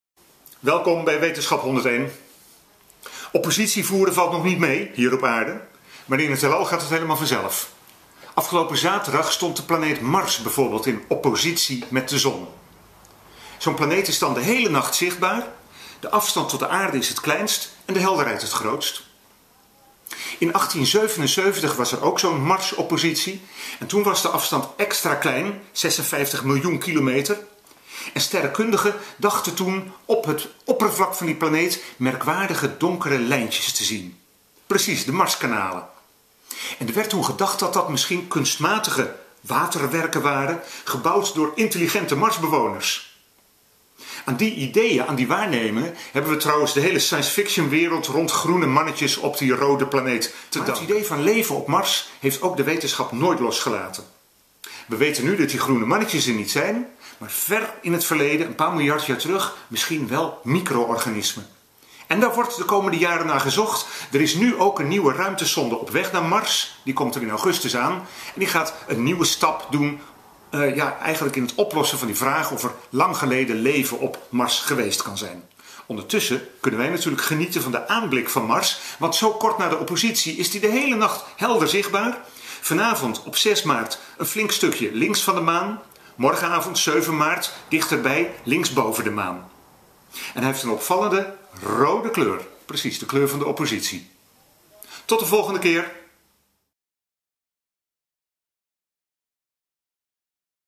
Rood is de kleur van de oppositie
Govert duikt in het verleden en vertelt hoe de speurtocht naar leven op Mars in 1877 begon met een extra gunstige oppositie van de Rode Planeet.